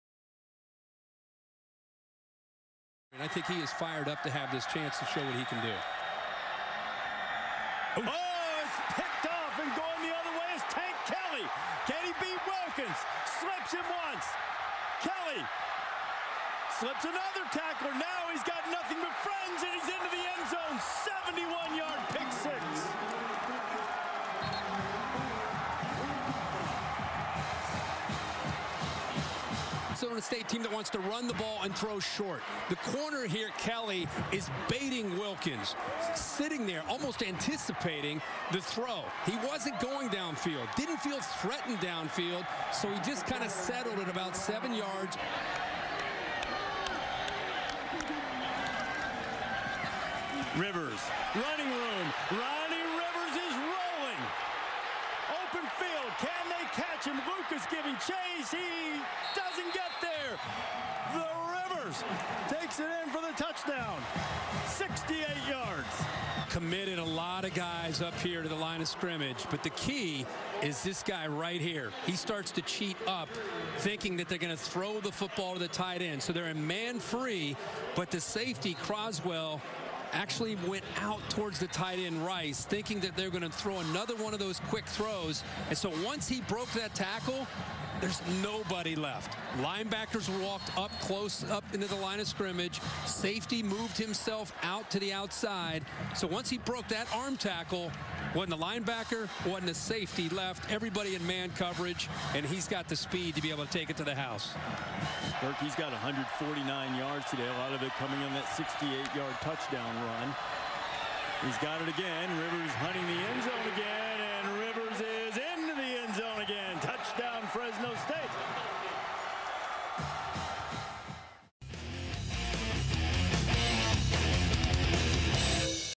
Highlights: Mitsubishi Motors Las Vegas Bowl - Fresno State Bulldogs vs Arizona State
Highlights from the 2018 Mitsubishi Motors Las Vegas Bowl, where the Fresno State Bulldogs defeated the Arizona State Sun Devils 31-20 at Sam Boyd Stadium in Las Vegas, NV